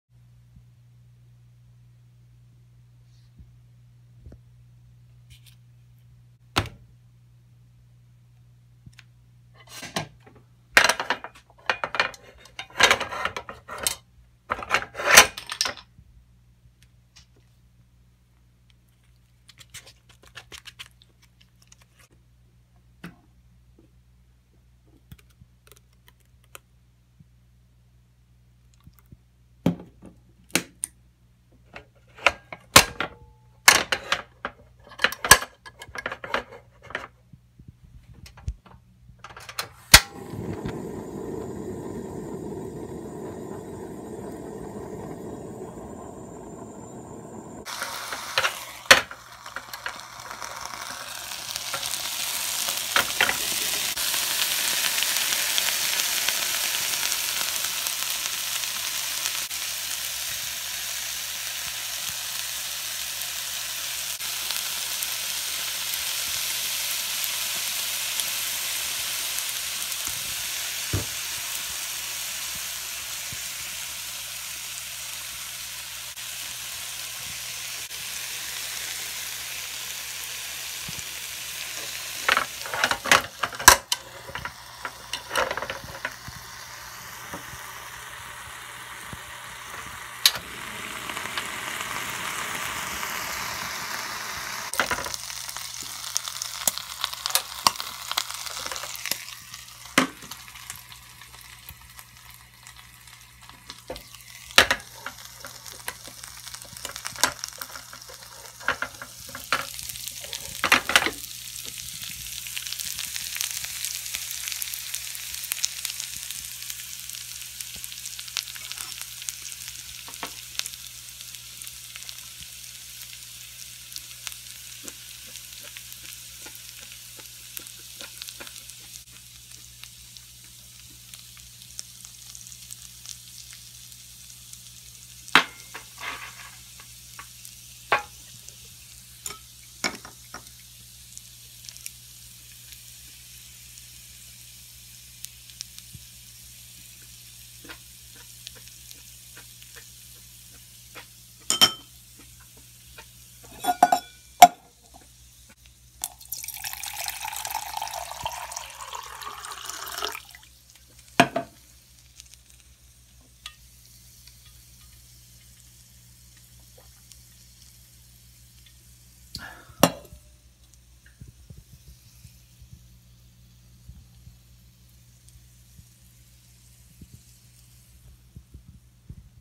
キャンプ飯 砂肝ガーリックバター醤油ホットサンド camp recipe ASMR
砂肝をガーリックバター醤油で焼いて食べる動画 HSM ホットサンドメーカー キャンプ飯 camp recipe ASMR

材料
砂肝
ニンニク
バター
醤油

アウトドアで使ってる料理アイテム
【トランギアラージメスティン】
【MiliCampメスティン】
【ポケットストーブ 】
【固形燃料】←100均より安くて高品質
【風防 】
【snowpeak包丁まな板 】
【snowpeakチタンマグ450】
【ホットサンドメーカー】